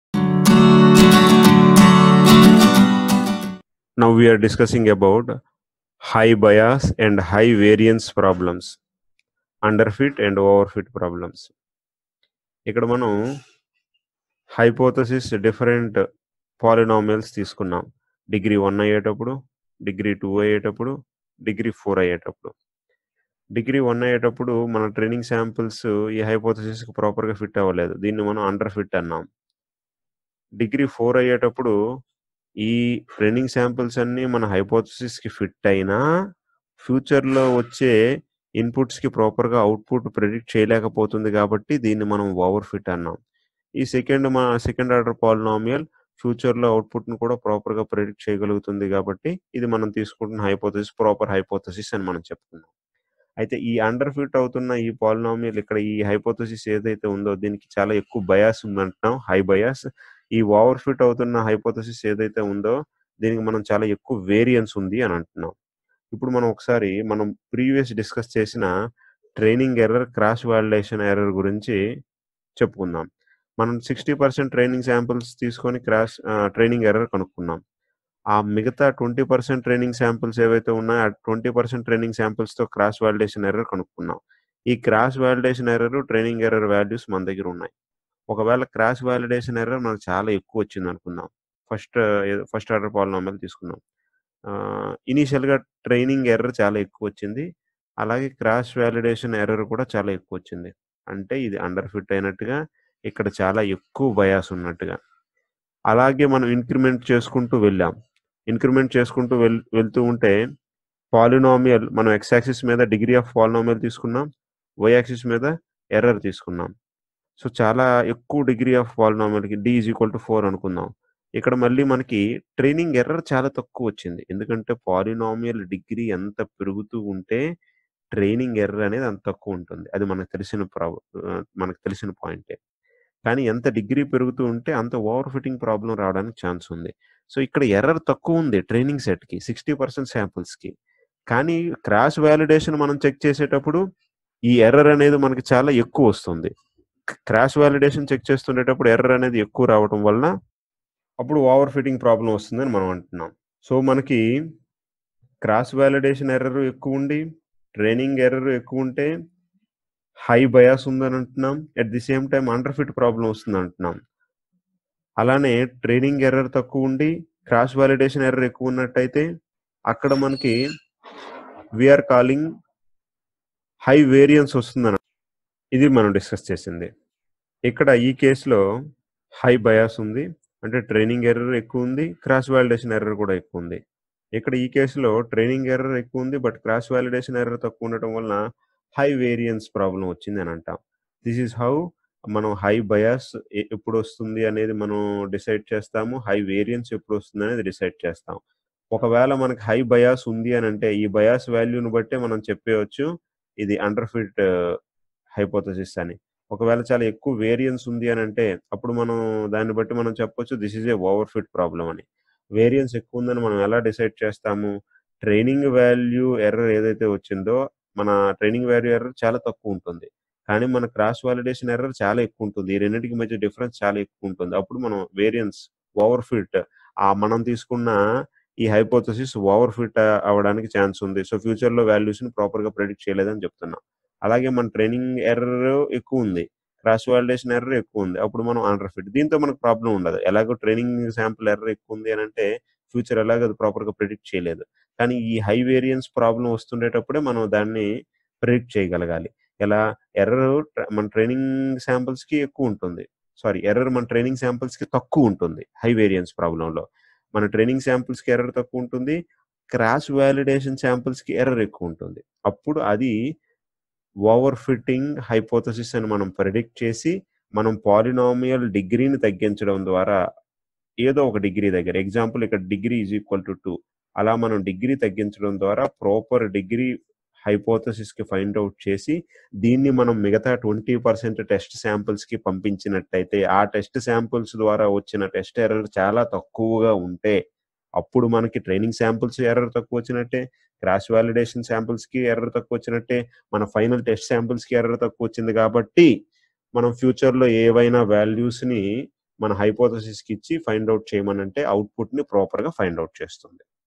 30-Bias and Variance
This lecture(12-07-20 Evening 6.00P.M class) is a part of zoom online classes to students on the concept of MACHINE LEARNING. This is recorded for the benefit of students. .